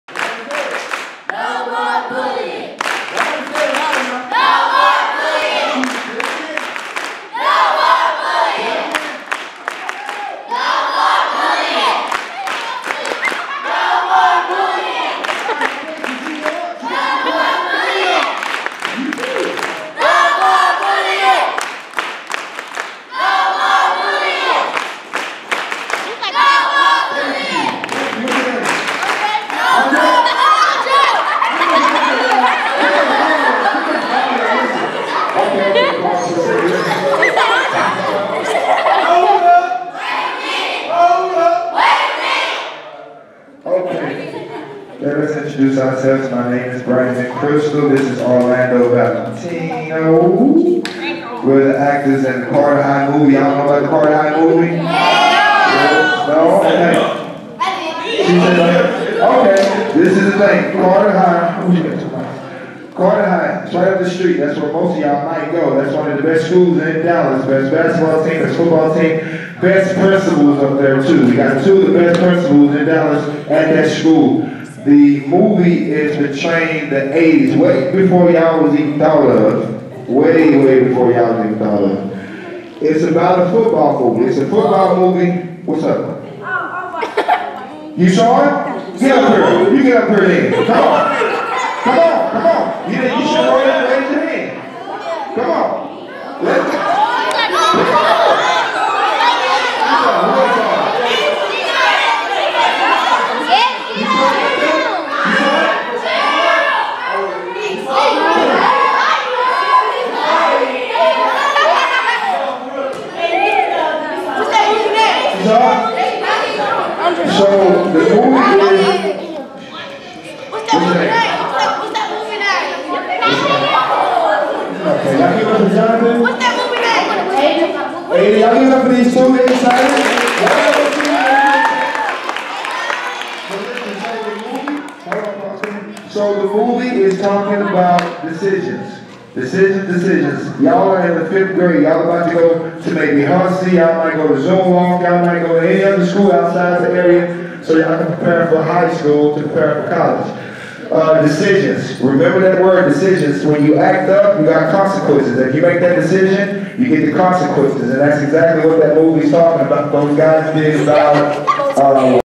In the Spirit Entertainment Foundation at Ronald McNair Elementary with Carter High Movie Actors
Lamar Strait and Orlando Valentino cast-mates from the Carter High Movie teamed with In the Spirit Entertainment Foundation Anti-Bullying campaign at Ronald McNair Elementary Dallas Texas...Anti-Bullying, go